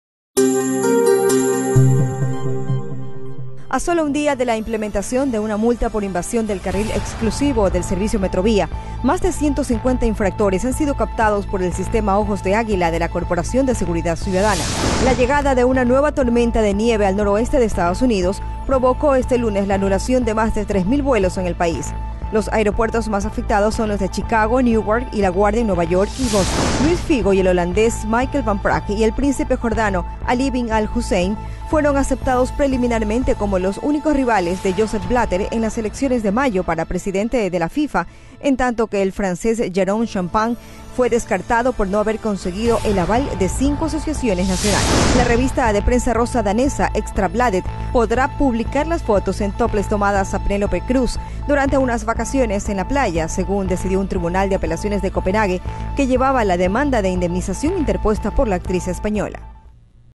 Al día en 60 segundos: 2 de febrero del 2015
Pese a que fue un día no laborable, y en consecuencia de tráfico moderado, decenas de conductores invadieron ayer el carril exclusivo de la Metrovía, en diferentes sectores de Guayaquil. Las imágenes de los infractores fueron capturadas por las cámaras de la Corporación para la Seguridad Ciudadana de Guayaquil (CSCG), en el primer día de estos controles.